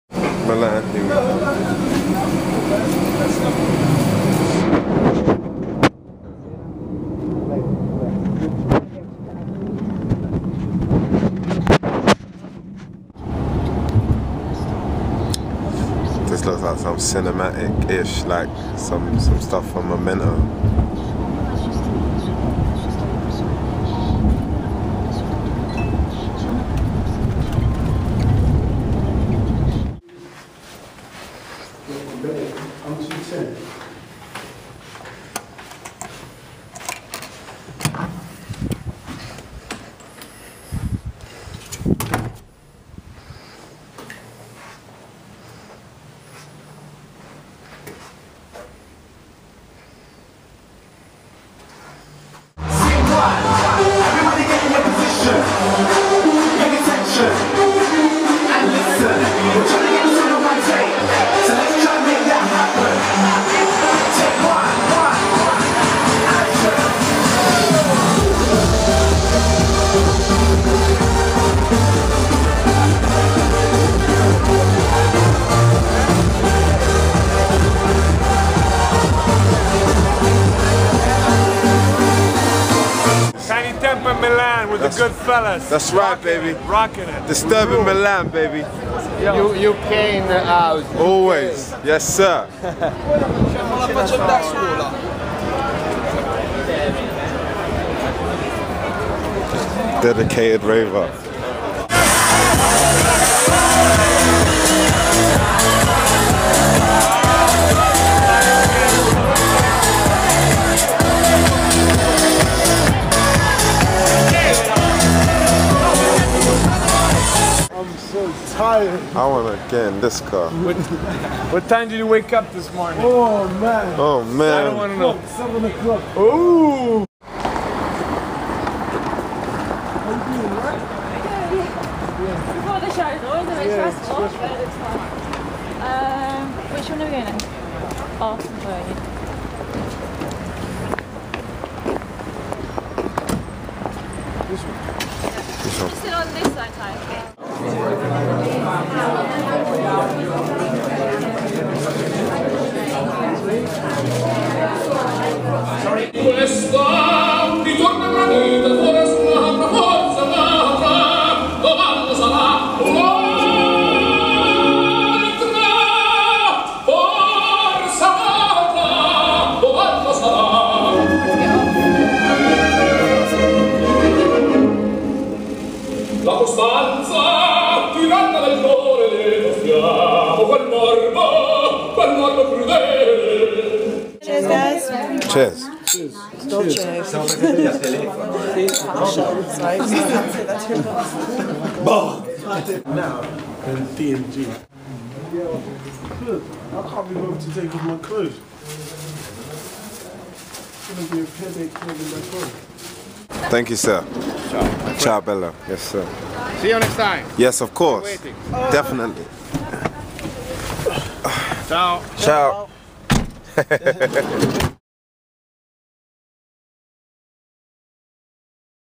Tinie Tempah | Disturbing London TV: DisturbingMilan with Dolce & Gabana
Check out what happened when Tinie Tempah went to the Dolce & Gabana fashion show in Milan, Italy..